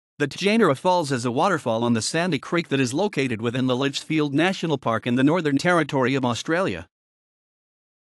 Tjaynera Falls
The Tjaynera Falls is a waterfall on the Sandy Creek that is located within the Litchfield National Park in the Northern Territory of Australia.